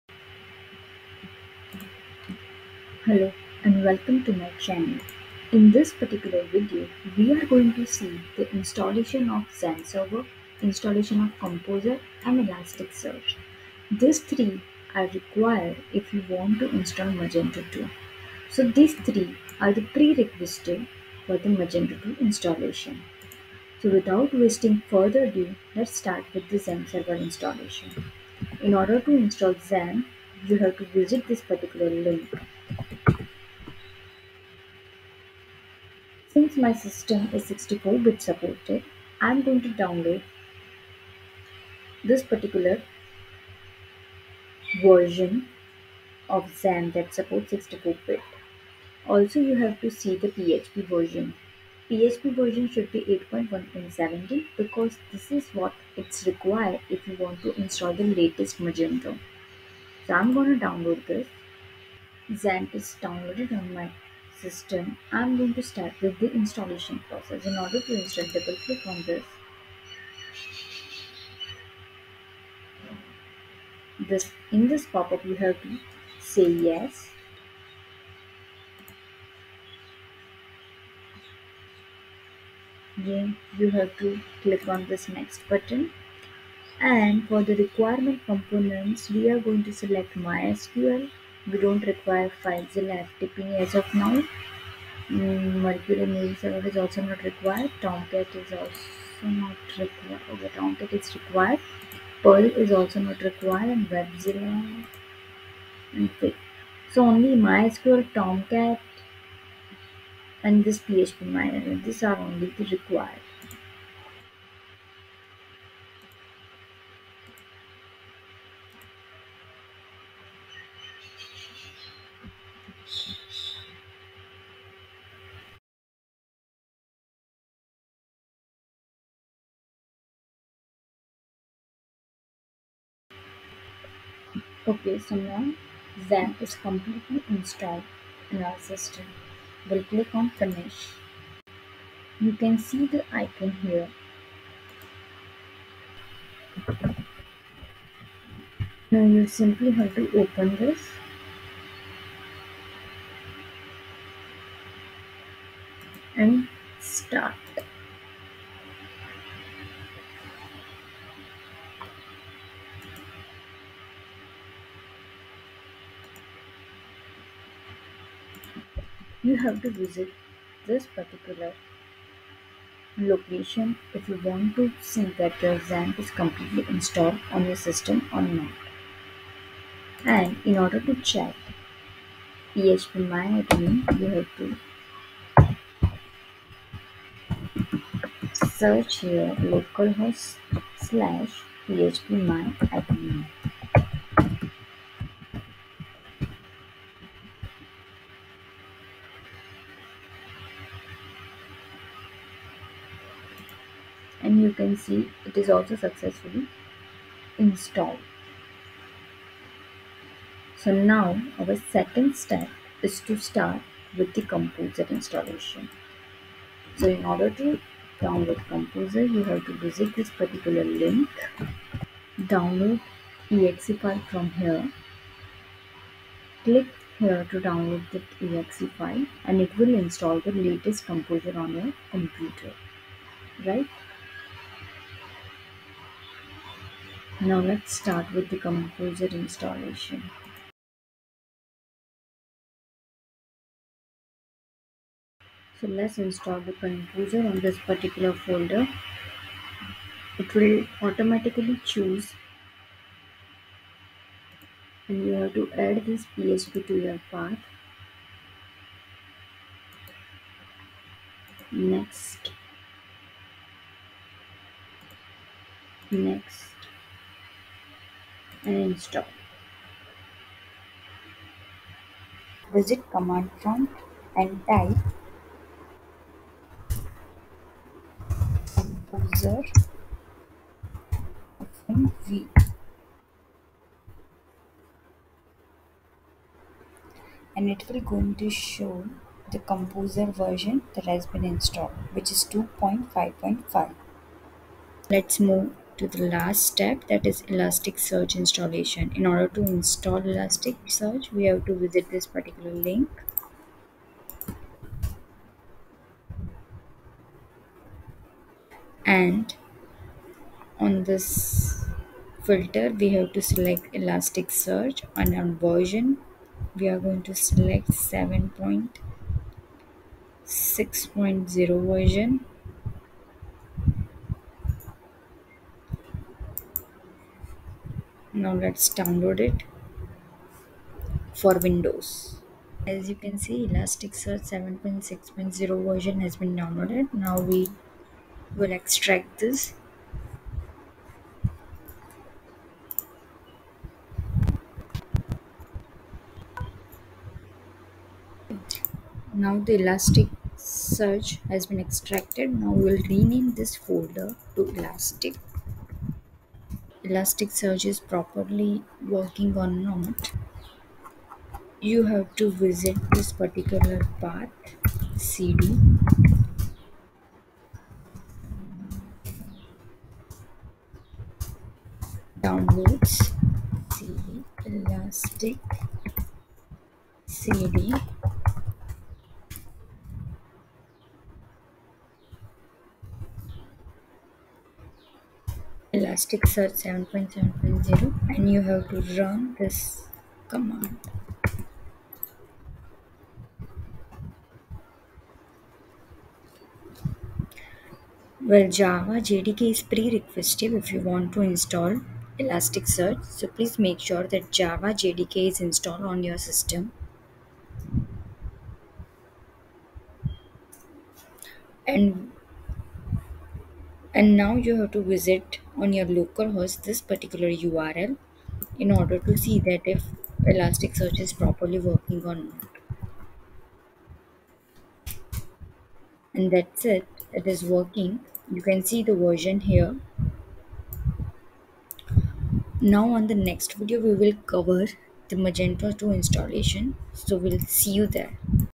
(Part-1)How to install prerequisite of Magento 2.4.6 on Windows 10 (xampp, composer, elasticsearch)
For windows 10 perform below steps to install xampp , composer and elastic search
1)Install xampp server

3)Install elastic search (Java JDK is prerequisite for elastic search installation

To run elastic search run below mentioned command on elastic search folder
.\bin\elasticsearch.bat

To check elastic search running properly hit below link on browser